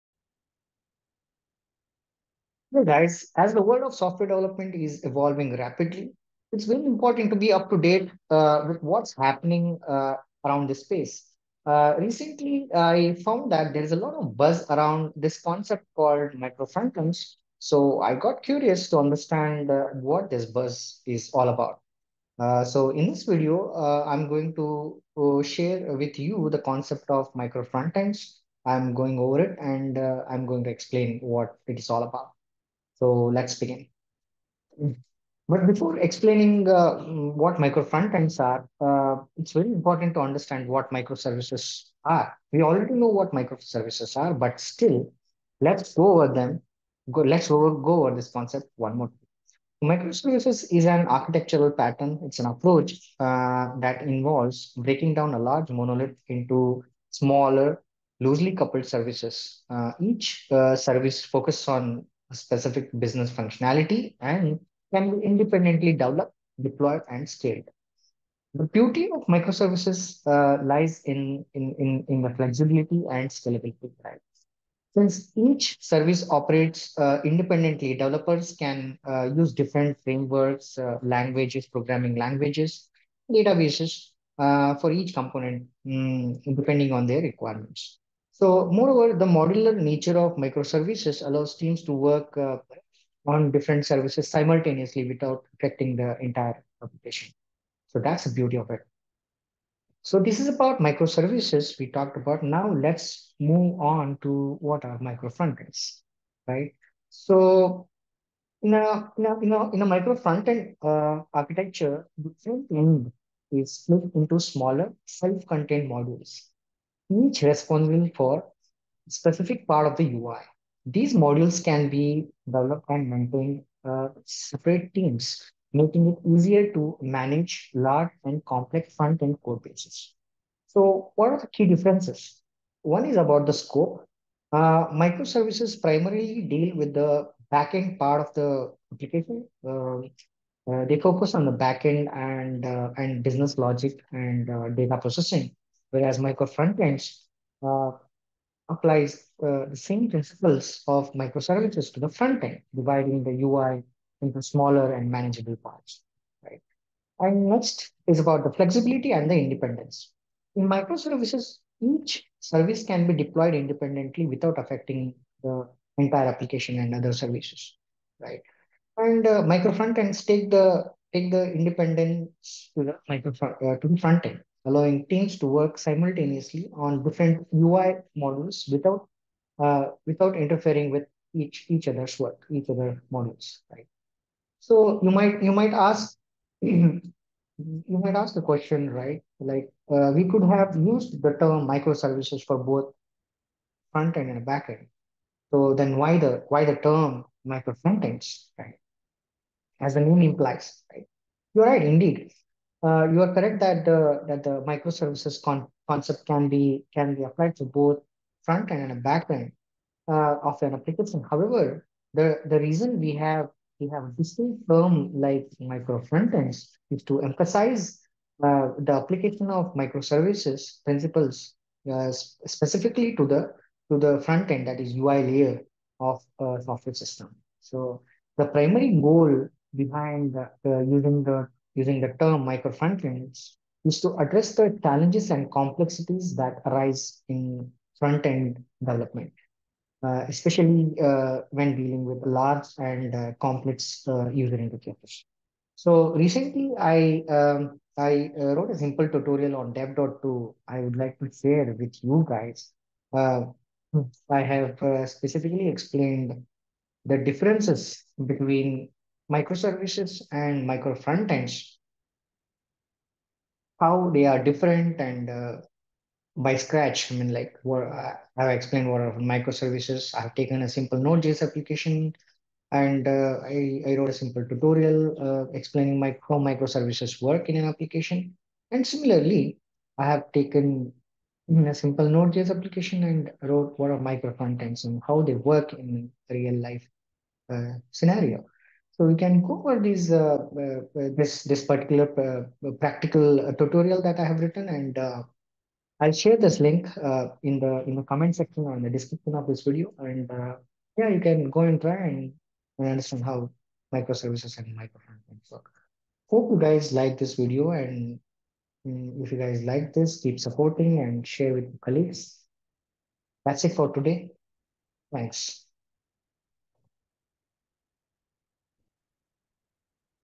Microservices vs. Micro Frontends
New to software architecture? 🤔 This beginner-friendly video explains Microservices and Micro Frontends in simple terms. Learn the key differences, benefits, and use cases of each approach. Discover which one suits your projects best!